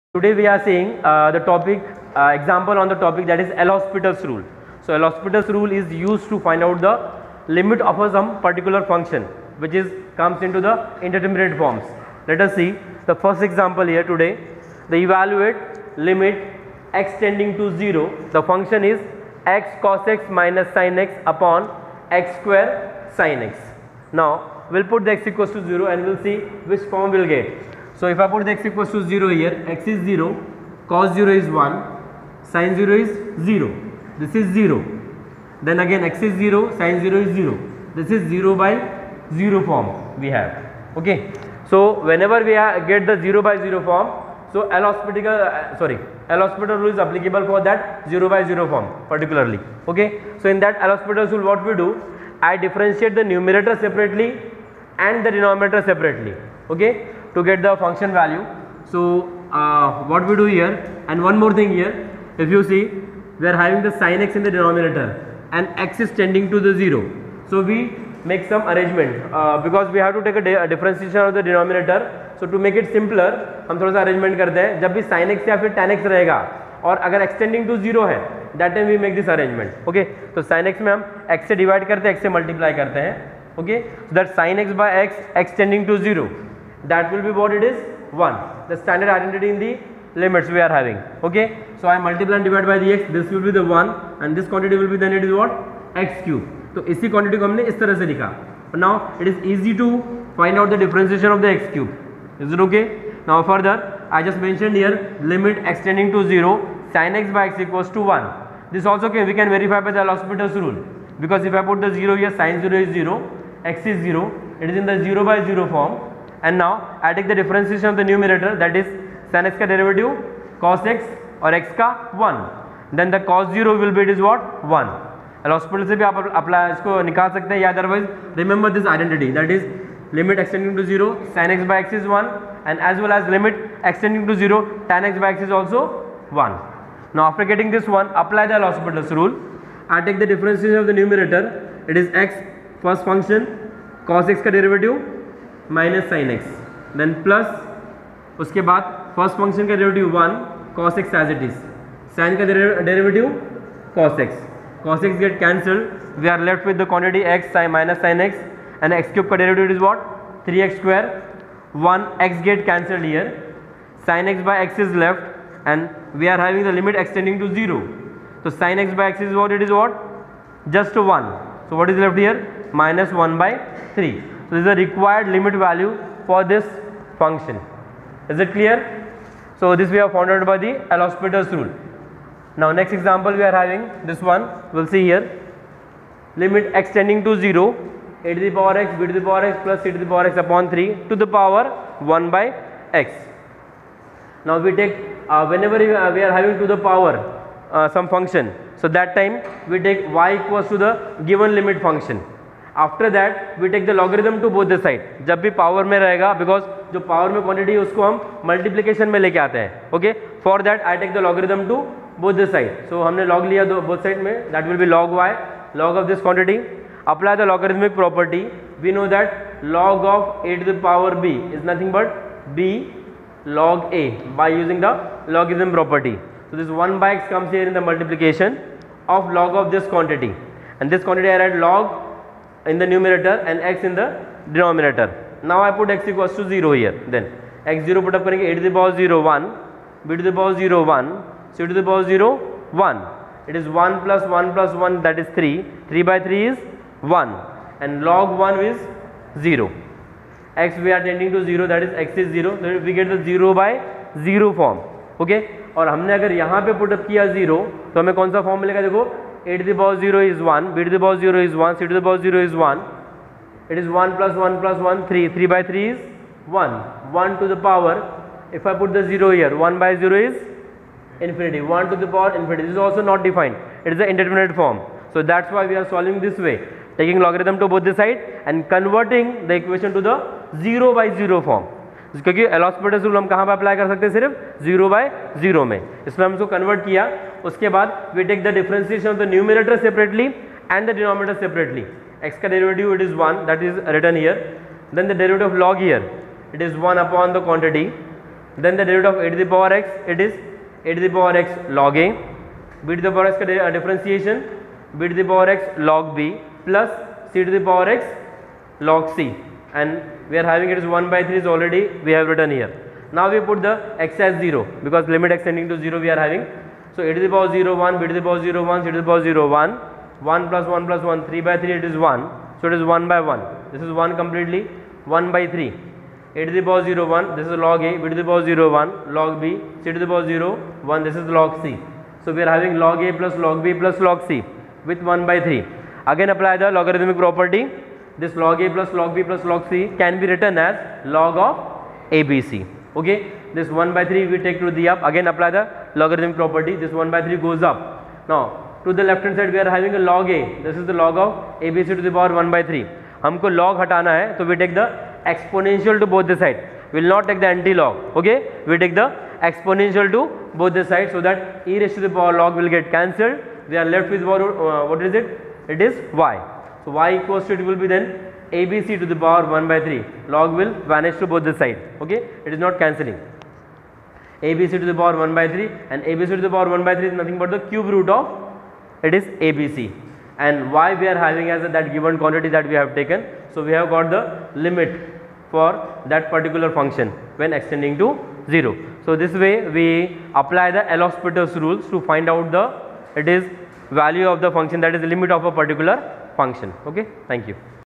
Indeterminate Forms and L'Hospital's Rule (Example)
This video includes solution of indeterminate forms by using L'Hospital's rule. In the first example, we use the standard identity lim x tends to 0 sinx/x=1 and reduced given rational function into simple form and applied L'Hospitals rule to solve it. Second example is of the form 1^infinity, we induce it into 0/0 form by taking exponential to both the side and solve it by using L'Hospital's rule.